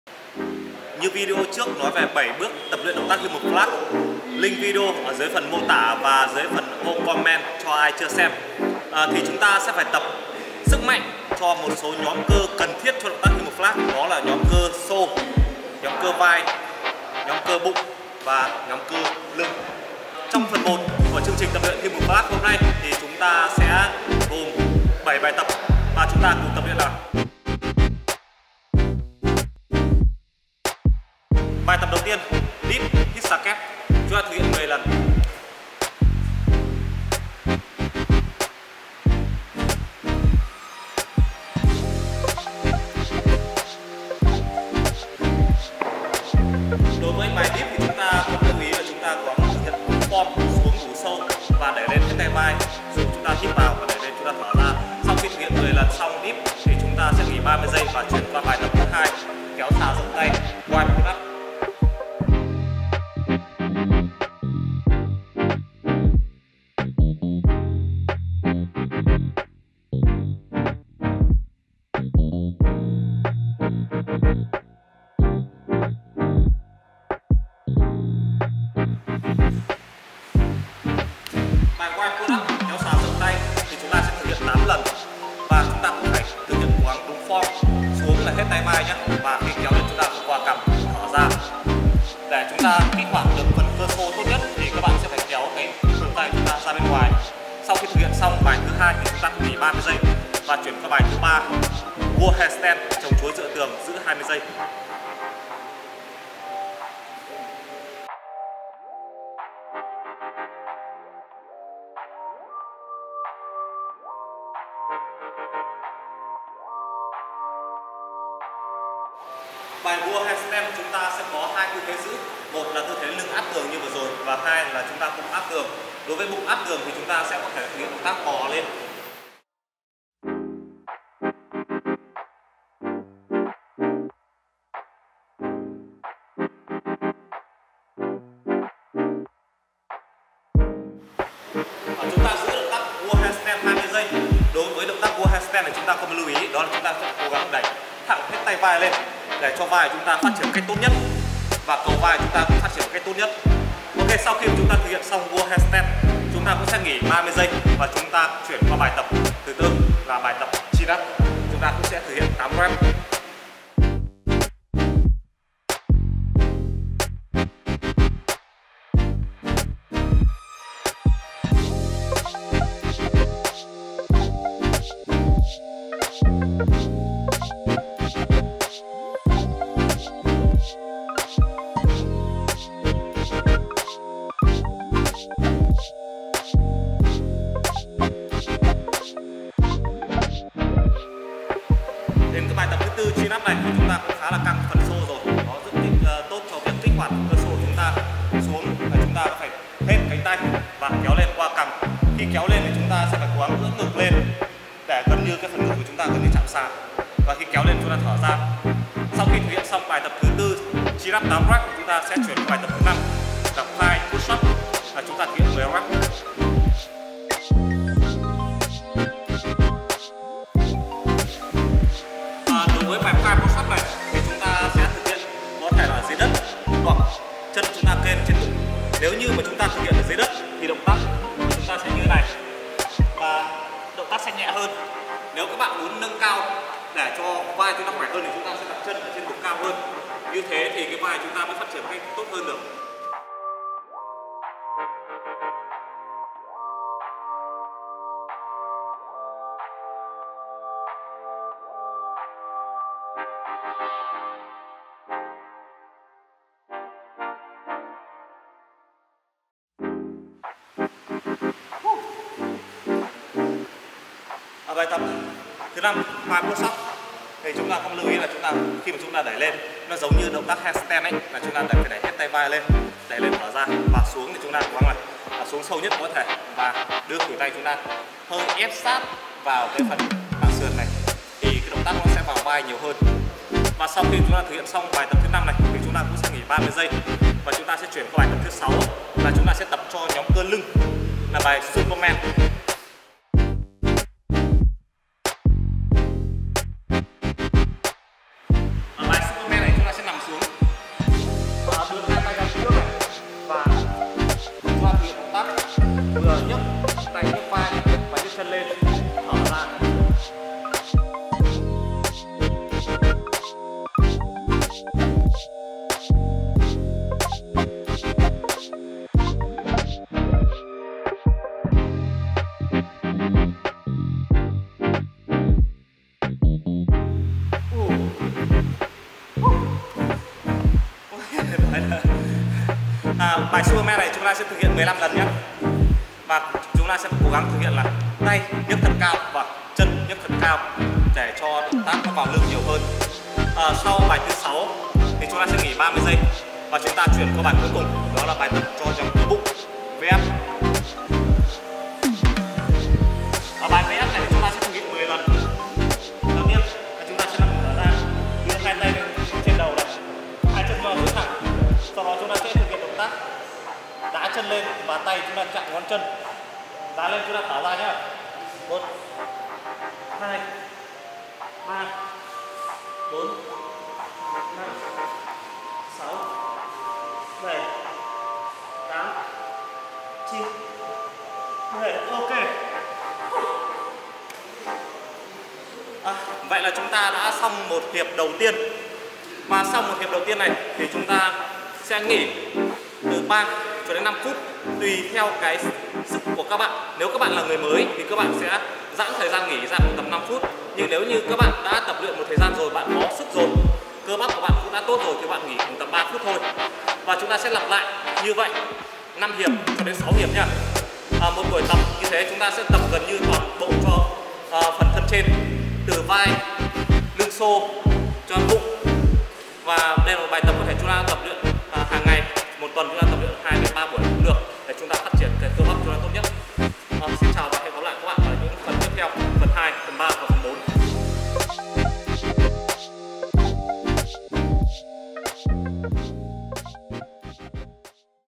Hướng dẫn HUMAN FLAG | Phần 1 - Tạo sức mạnh nền tảng cho người tập Calisthenics
Human flag (HF) có độ khó level 8, được coi là động tác siêu nhân và rất nhiều người muốn tập.
PHẦN 1 trong chương trình hướng dẫn tập human flag, mọi người sẽ phải tập luyện để xây dựng được sức mạnh nền tảng của cơ bắp.

Human flag tutorial by Hùng chủ tịch VNSWCF

- Tổ chức Thể dục đường phố Việt Nam